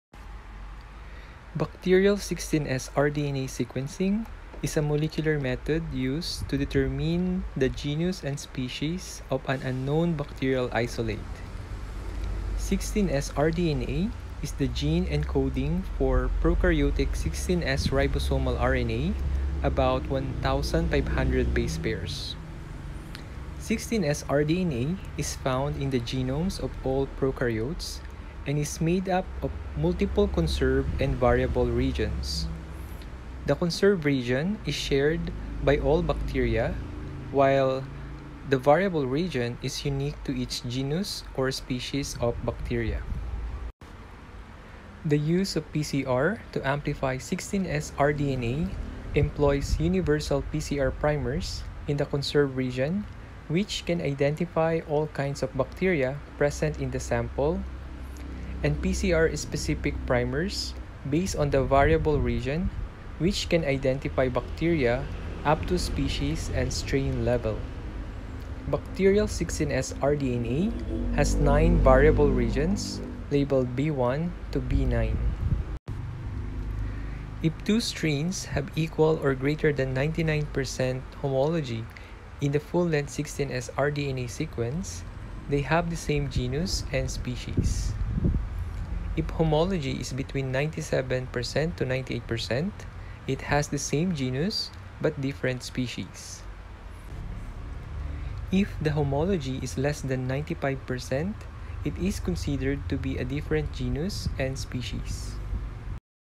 How To Interpret 16S rDNA Sequence Homology 🧬🧫
16S rDNA Sequencing: Standard DNA Marker for Molecular Identification of Bacteria

Bacterial 16S rDNA sequencing is a molecular method used to determine the genus and species of an unknown bacterial isolate. 16S rDNA is the gene encoding for prokaryotic 16S rRNA, with a length of about 1500 bp.  It is found in the genomes of all prokaryotes, and is made up of multiple conserved and variable regions (Zheng et al., 2021). The conserved region is shared by all bacteria, and there are no significant differences between them. While the variable region varies between bacteria and is unique to each genus or species of bacteria (Reller et al., 2007).

The use of PCR to amplify 16S rDNA employs universal PCR primers in the conserved region, which can identify all kinds of bacteria present in the sample and PCR-specific primers based on the variable region, which can identify bacteria up to species and strain level (Sontakke et al., 2009). Bacterial 16S rDNA has nine variable regions labeled V1-V9 (Jumpstart Consortium Human Microbiome Project Data Generation Working Group, 2012).

In general, if two strains have equal or greater than 99% homology in full length 16S rDNA sequence, they are considered the same bacteria. If homology is 97%-98%, it is considered to be a different species of the same genus. If the homology is less than 95%, it is considered to be a different genus (Zheng et al., 2021).

References
1. 16S rDNA Amplification.
2. Jumpstart Consortium Human Microbiome Project Data Generation Working Group (2012) Evaluation of 16S rDNA-Based Community Profiling for Human Microbiome Research. PLoS ONE, 7, e39315
3. Reller BL, Weinstein MP, Petti CA (2007) Detection and Identification of Microorganisms by Gene Amplification and Sequencing. Clin Infect Dis, 44, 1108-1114
4. Sontakke S, Cadenas MB, Maggi RG, Diniz PPVP, Breitschwerdt EB (2009) Use of Broad Range 16S rDNA PCR in Clinical Microbiology. J Microbiol Methods, 76, 217-225
5. Zheng J, Zhu J, Chen B, Chen LX, Gao T, Chen XP, Li FY (2021) Application of 16S rDNA Sequencing Technology in the Analysis of Pathogenic Bacteria in Sputum of Severe Pneumonia. Adv Microbiol, 11, 157-164